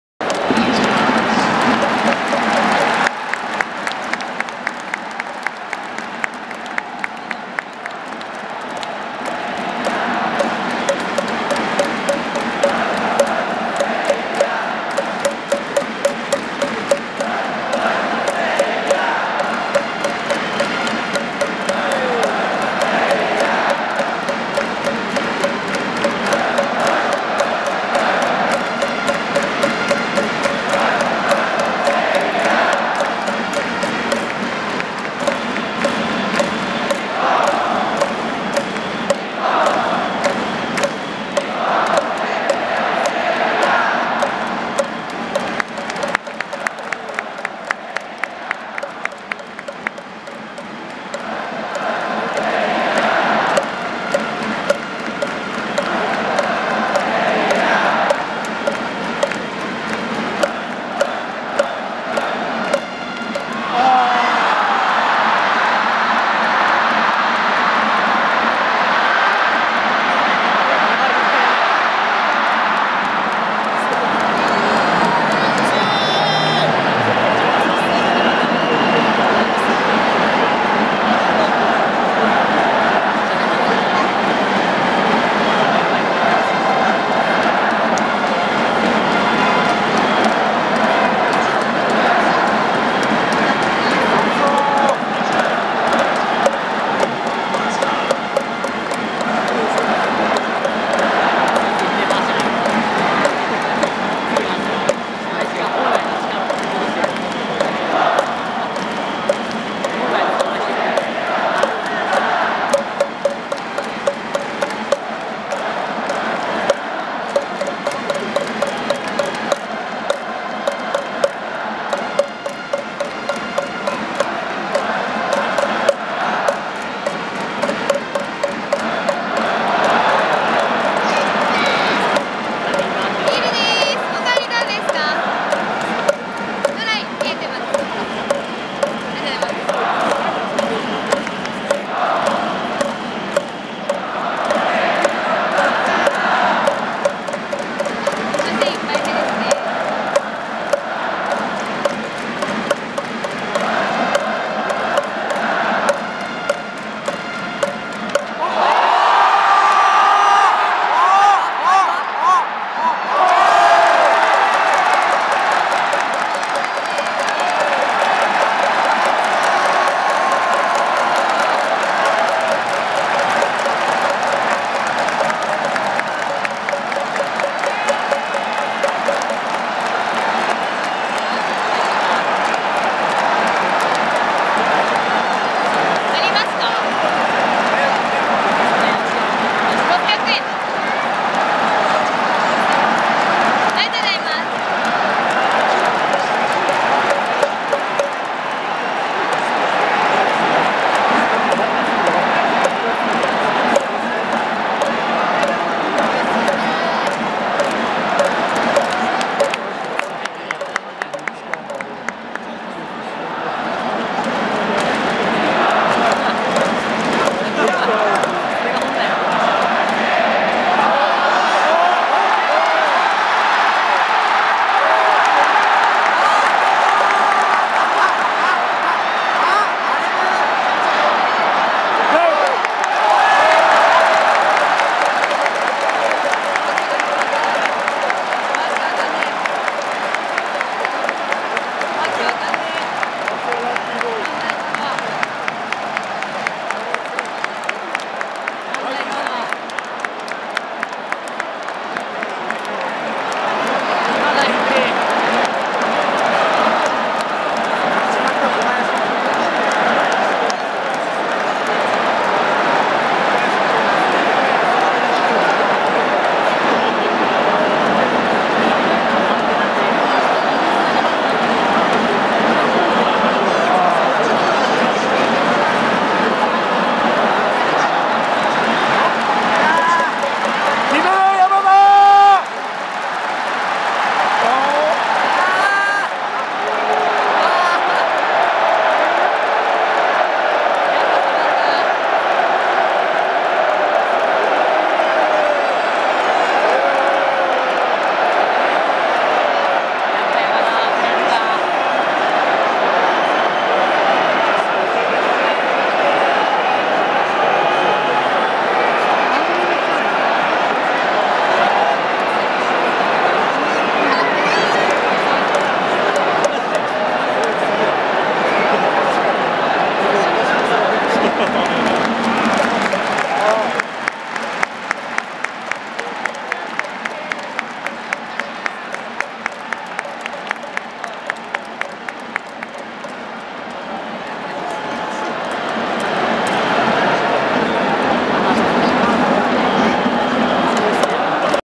6回裏／鈴木誠也（三ゴロ）→松田宣浩（左２）→小林誠司（三安）→山田哲人（敬遠）／2017.3.15 イスラエル×日本 2017WBC東京2次ラウンド東京ドーム
6回裏／鈴木誠也（三ゴロ）→松田宣浩（左２）→小林誠司（三安）→山田哲人（敬遠）／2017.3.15　イスラエル×日本　2017WBC東京2次ラウンド東京ドーム